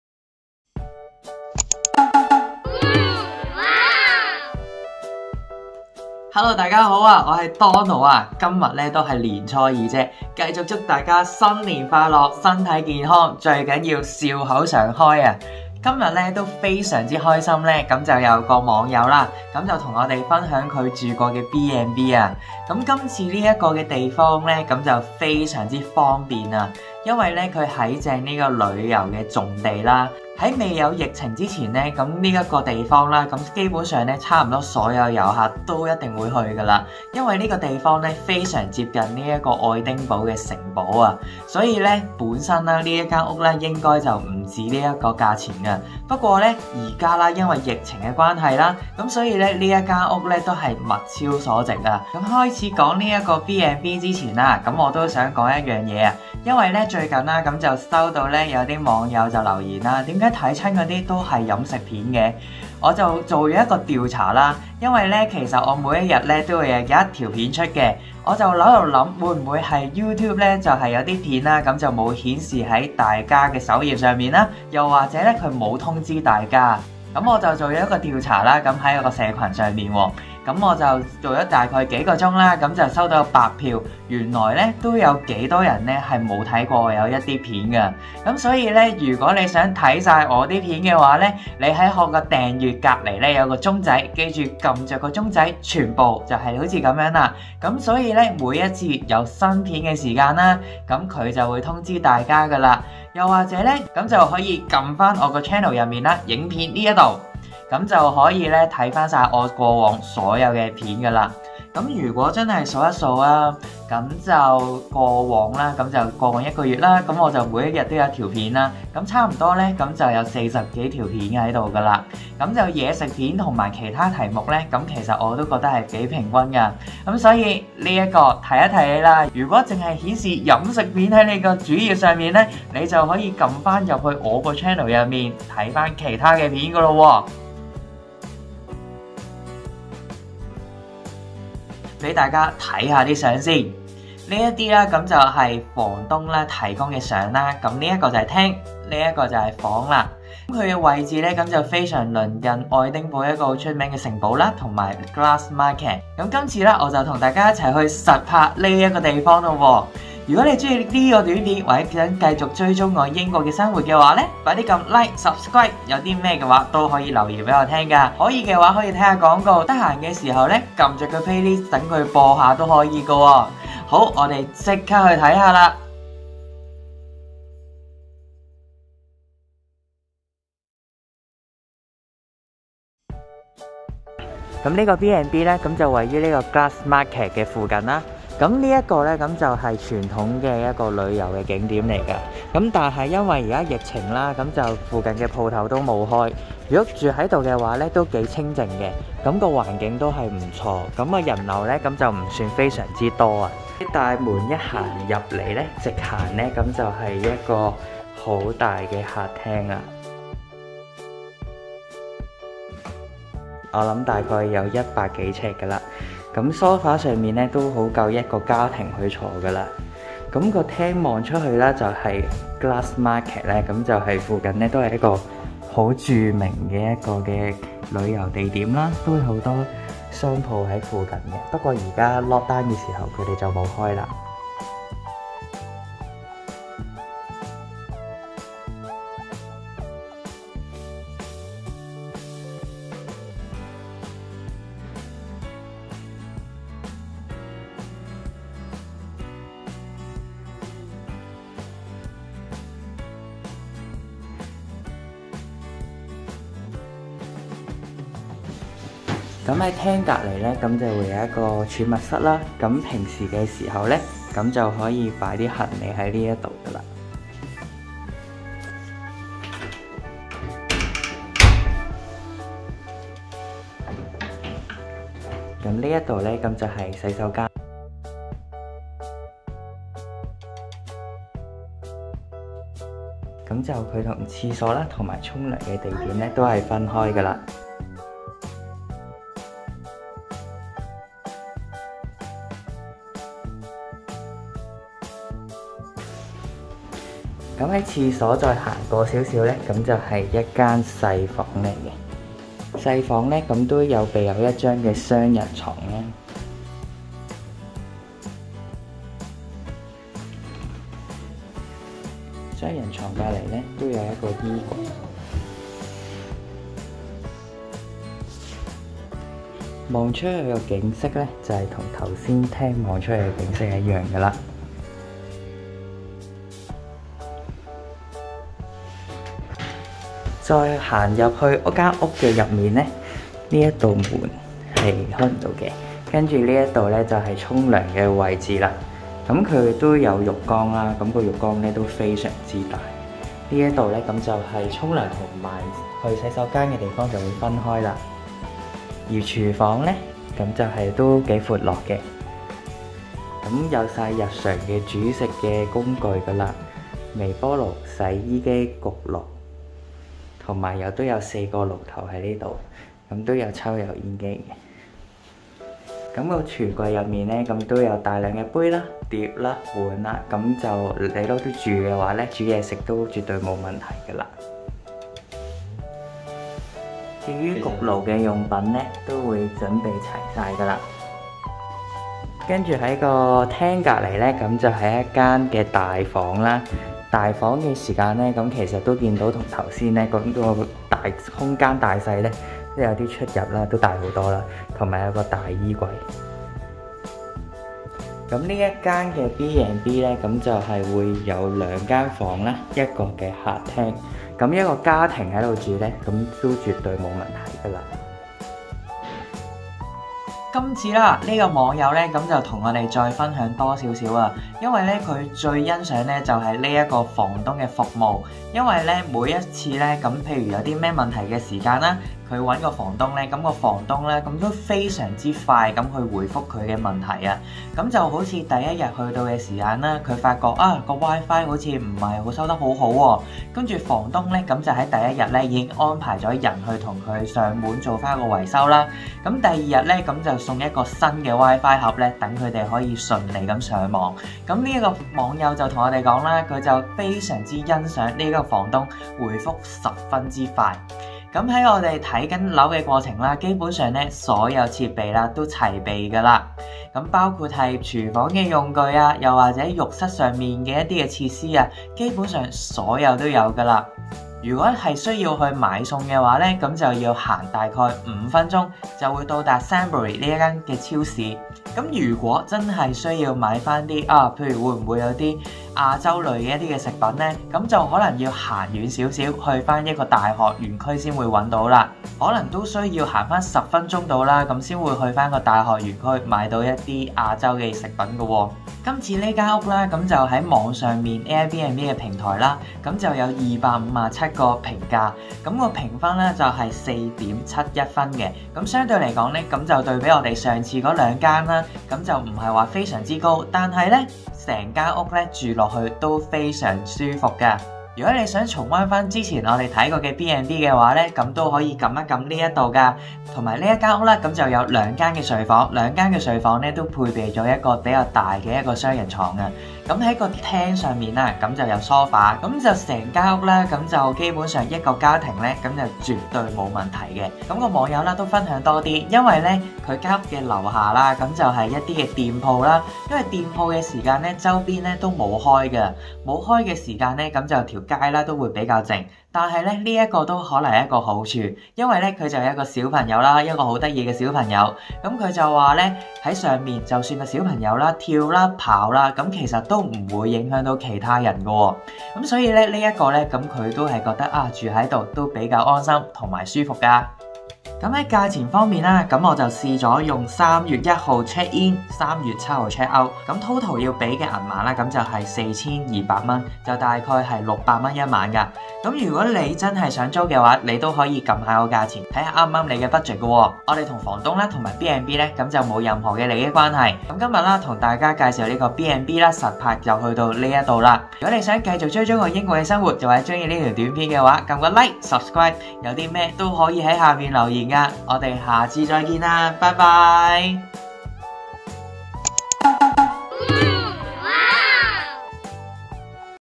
英國生活 - 港人暫落腳點 - AIR B&B實拍 EP3 - 評分4.71~ 2房大廳~ 市中心 每晚只需$5XX 附預訂連結及租客評語 - BNO
🎉如果你想請我飲杯咖啡, 食個蛋撻❣️, 可按

即刻申請電話卡 (有5磅現金券) 提提大家, 當月申請+啟用先有5磅現金券

英國生活 - 由於太多網友分享遇過中伏B&B, 所以早前向網友呼籲, 如果租到好既B&B, 就可以比我拍低佢, 再介紹比下一位, 將中伏機會大大減低。加上LOCKDOWN期間, 部份酒店不予預訂, 能成功預訂的B&B就十分重要。